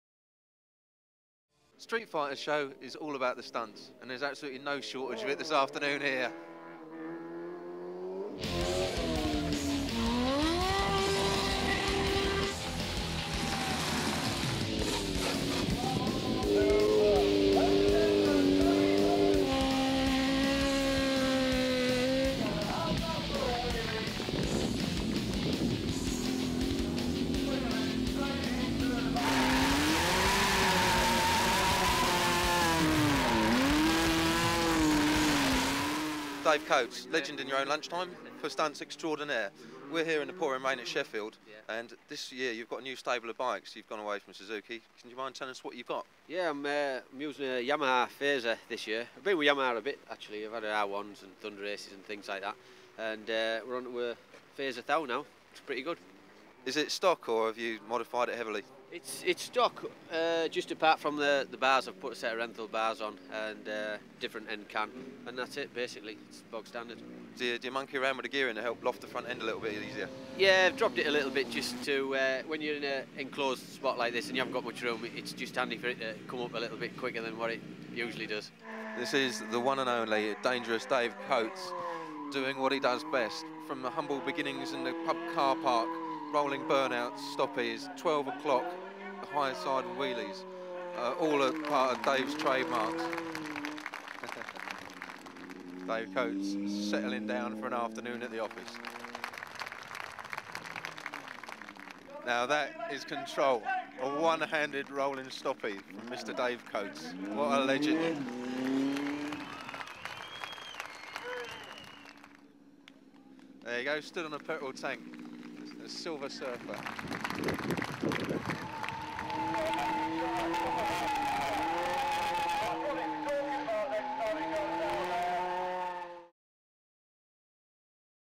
Streetfighter Stunt Show - Sheffield Arena
Stuart Evans is at the Sheffield Arena checking for the 3003 Streetfighter Show and speaking to the legend, Dangerous Dave Coates. Dave talks briefly about his humble beginnings as a stunt rider, before showing us some of the stunt tricks that earned him the nickname - Dangerous Dave Coates.